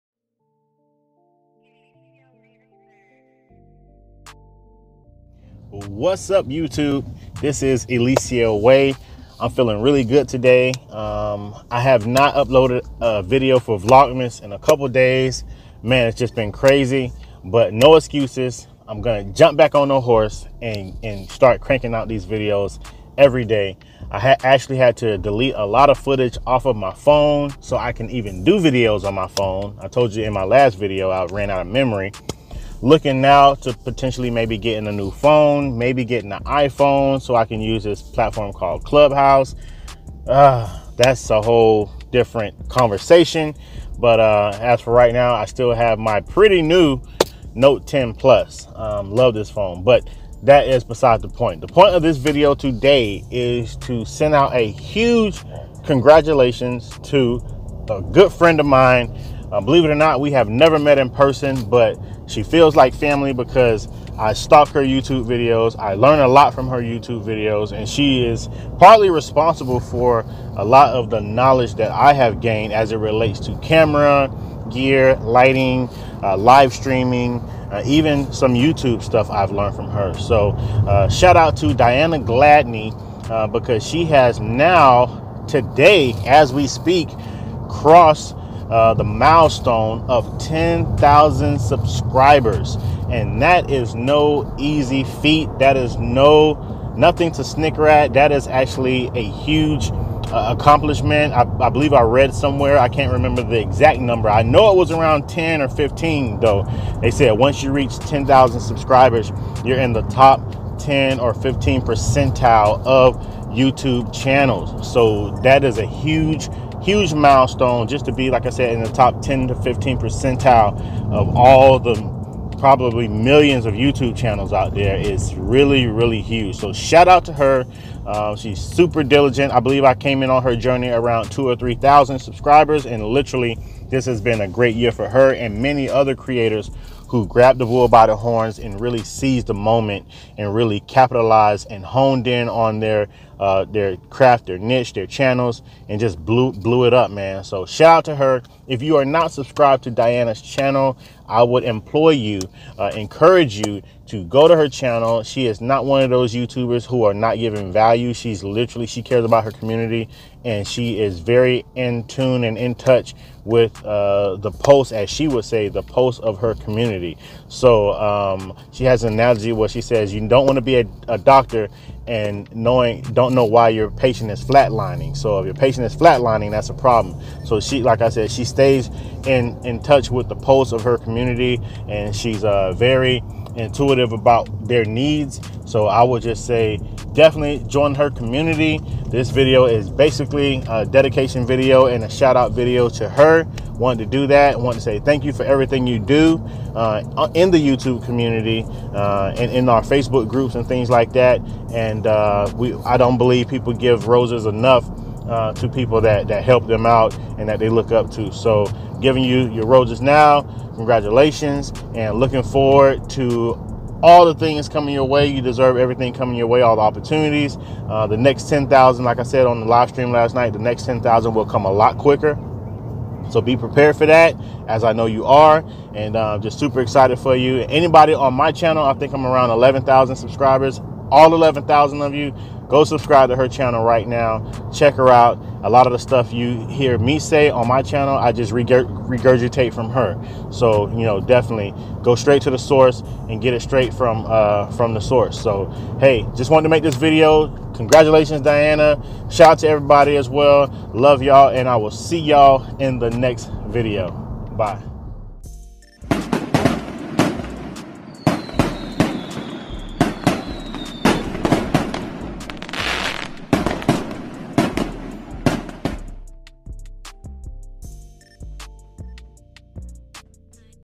Congratulations Diana Gladney on 10,000 YouTube Subscribers!!!
Huge congratulations to Diana Gladney  for reaching 10,000 subscribers on YouTube. I've learned a lot from you over the last year by watching your videos. Super excited for you reaching such a huge milestone. What an accomplishment! Thanks for everything!